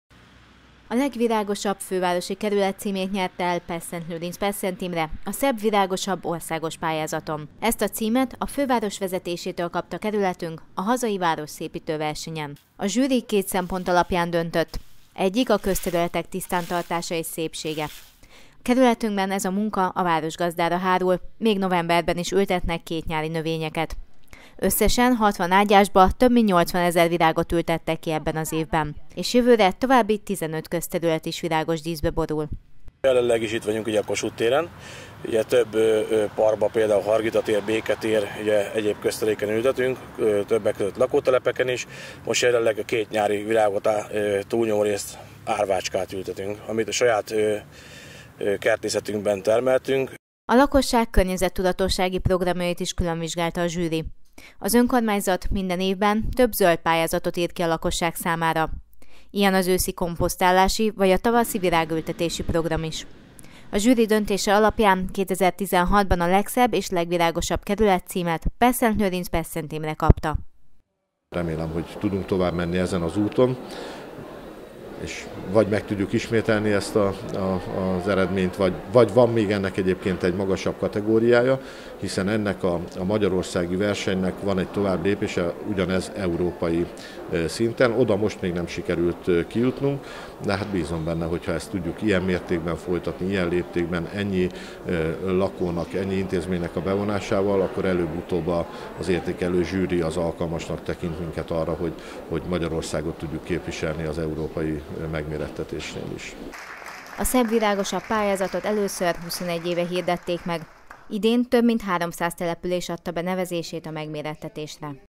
Szebb és virágosabb kerület
Pestszentlőrinc-Pestszentimre idén elnyerte a fővárosi kerületek kategóriában a Szebb-virágosabb pályázat fődíját.